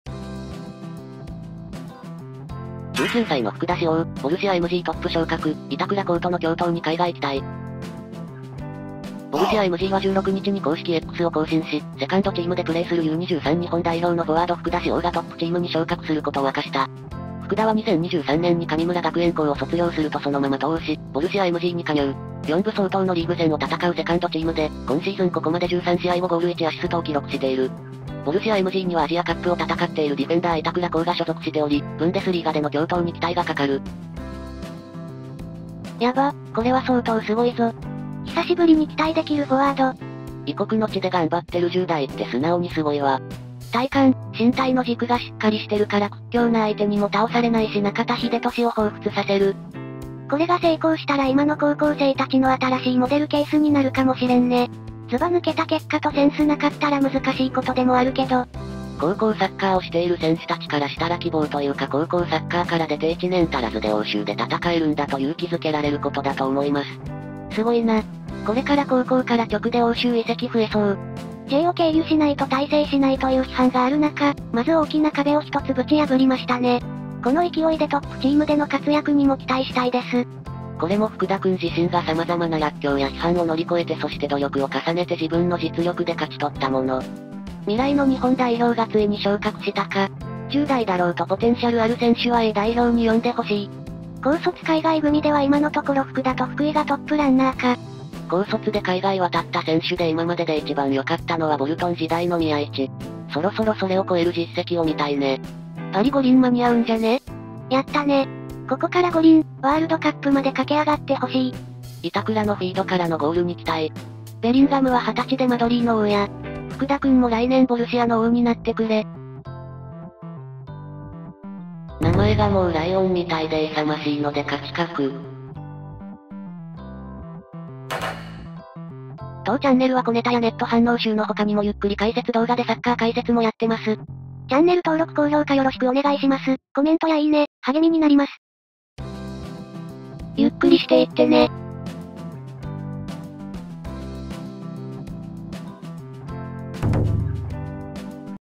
【朗報】19歳の福田師王、ボルシアMGトップ昇格！板倉滉との共闘に海外期待！
ボルシアMGは16日に公式Xを更新し、セカンドチームでプレーするU-23日本代表のFW福田師王(19)がトップチームに昇格することを明かした。
ボルシアMGにはアジアカップを戦っているDF板倉滉が所属しており、ブンデスリーガでの共闘に期待がかかる。

今回はX(旧Twitter)や2chなど様々なネットの反応・コメントを集めた動画になります。
h ttps://news.yahoo.co.jp/articles/3d8238bcfa574e212b9cb495f2e140bde71ac676

このチャンネルはサッカー関連の動画を毎日更新、投稿しています。